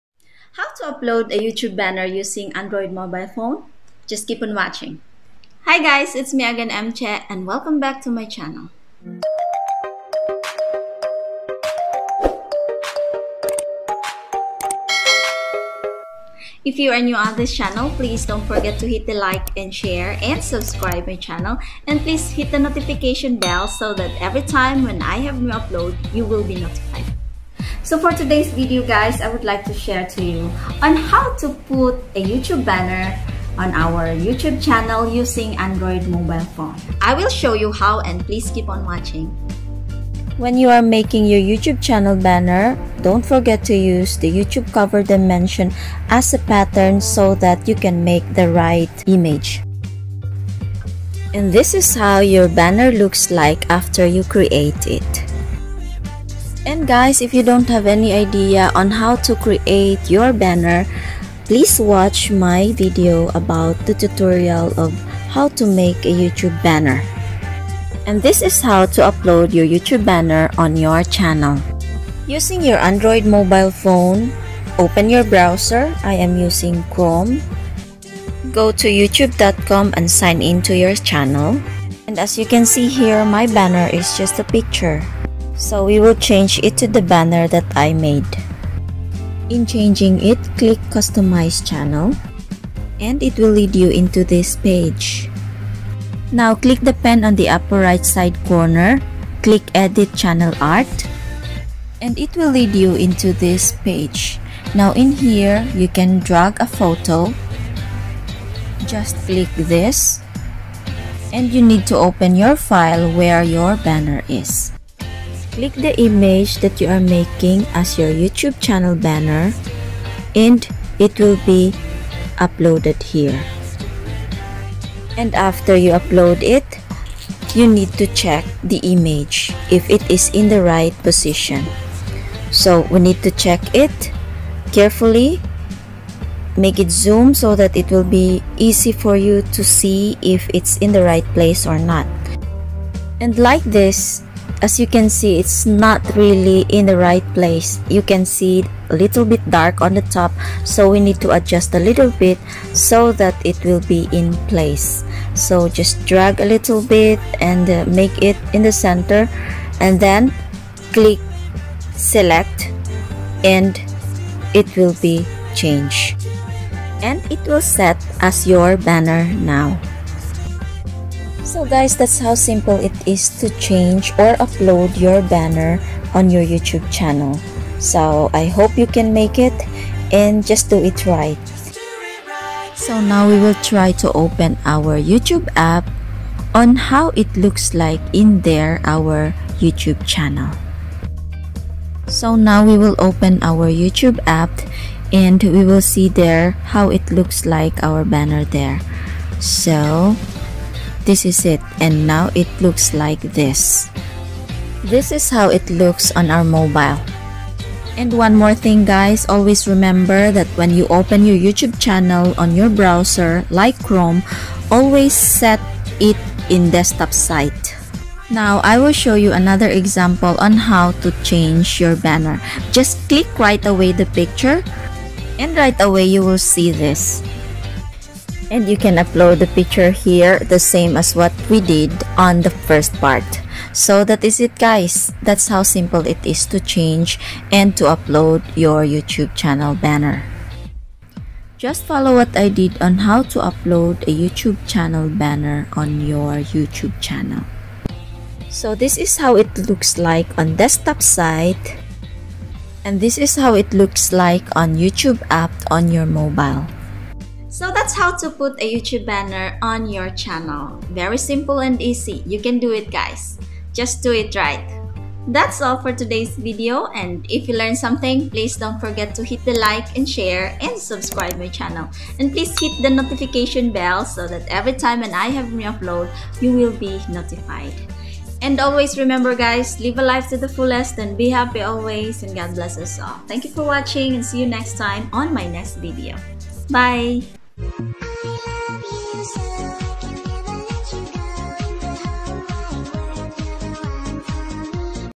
HOW TO UPLOAD YOUTUBE CHANNEL BANNER USING ANDROID MOBILE PHONE
A video tutorial on how to upload a YouTube channel banner using your Android mobile phone.